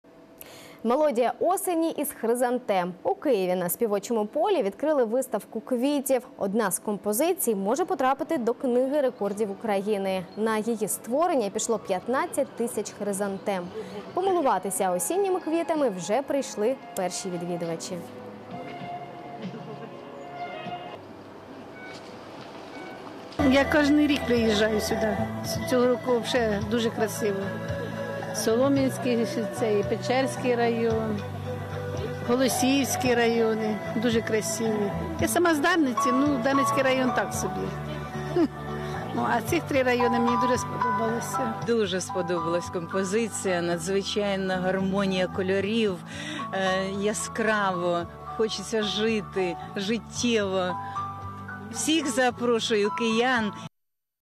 У Києві на Співочому полі відкрили виставку квітів
Одна з композицій може потрапитити в книги рекордів України. На її створення пішло 15 тисяч хризантем. Помилуватися осінніми квітами вже прийшли перші відвідувачі.